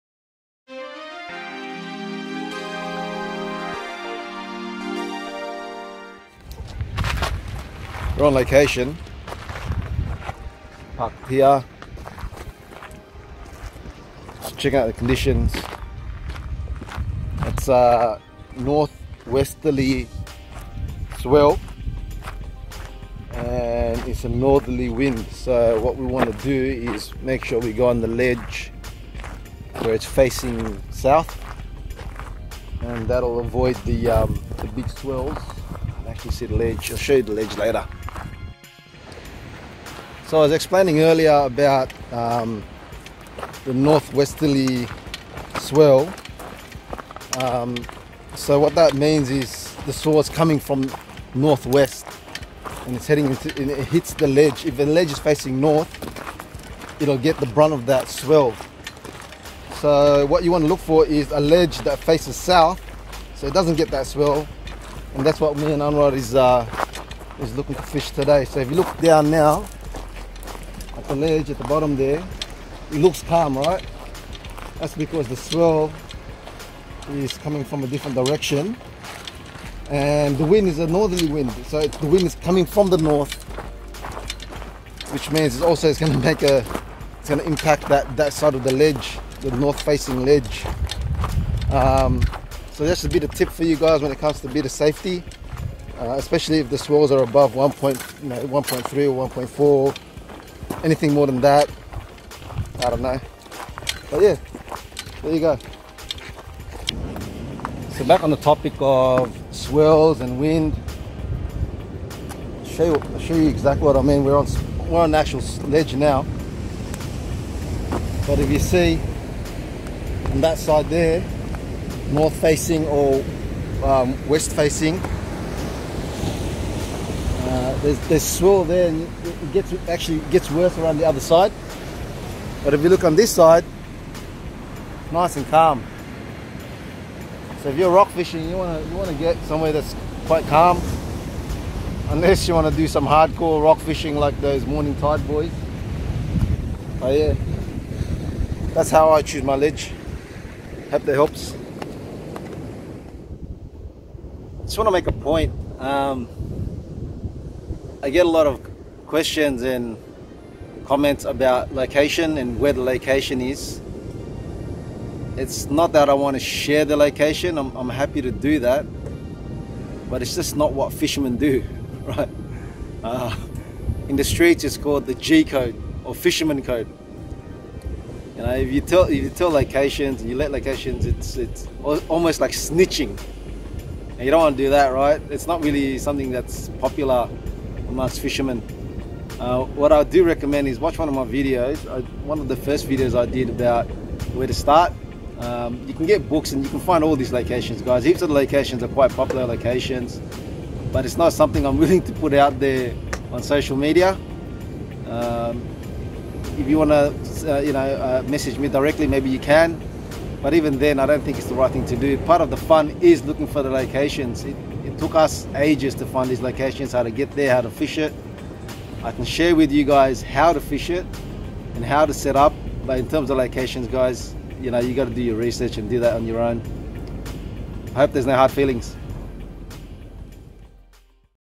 How to choose a rock fishing ledge [swell & wind]
Did you know that you can still go rock fishing if the swell and wind is high. I will explain how you can choose a safe rock fishing ledge based on the swell and wind.

I also talk about revealing fishing locations and the etiquette.

Hadi (a.k.a. Abu Tuna)